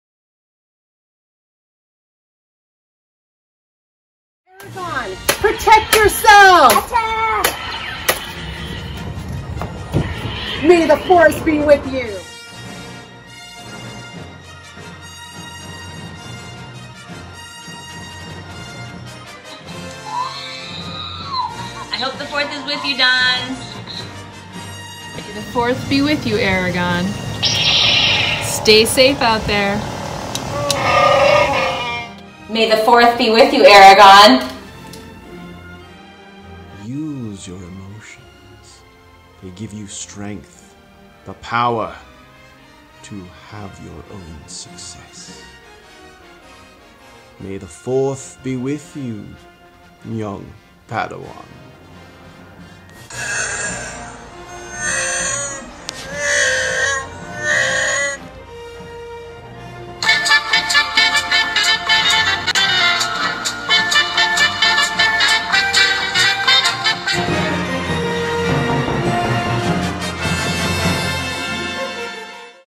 Aragon High School: May the Fourth Be With You 2020 Video
May the Fourth be with you, Dons!  Did you know that some of you AHS staff are one with the Force?  Check it out!"

This movie was created by Aragon Wellness Counselor Jillian Ma and features in order of appearance:
Josephine Ho, School Counselor
Lea Sanguinetti, School Counselor
Jillian Ma, Wellness Counselor
Nicole Elenz Martin, AP
Max Bernstein, Wellness Counselor
Vicky Reyes, School Counselor